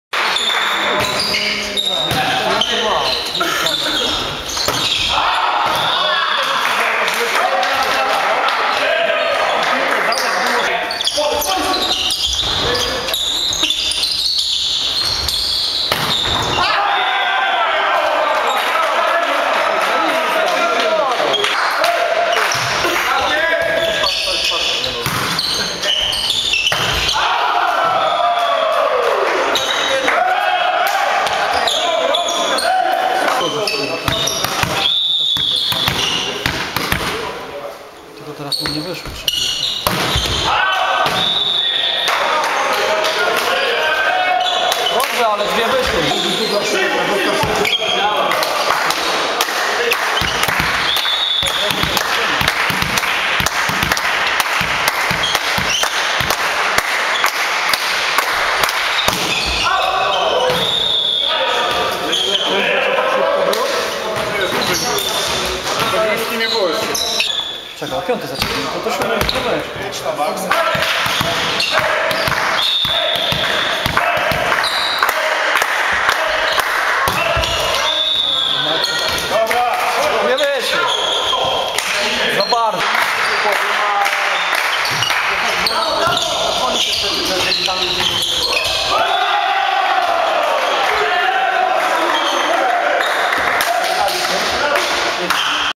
siatkówka Wołów-Głogów 2:3 highlight meczu 17.12.2011 Wołów Polska
najciekawsze akcje meczu w wykonaniu drużyny ULTRA OCHRONA TKKF Wołów, 3.liga DZPS:
2- Wojciech Kudyba
3- Tomasz Reczuch
4- Janusz Baranowski-libero
5- Arkadiusz Hajdrowski-kapitan drużyny
6- Sebastian Auguścik
7- Piotr Dacko
8- Michał Karauda
10- Rafał Elwartowski
11- Leszek Łapszyński
13- Piotr Siuciak
Trener: Krzysztof Olszewski
siatkówka piłka siatkowa Volleyball Poland

111217 Wołów-Głogów 2-3 highlights.avi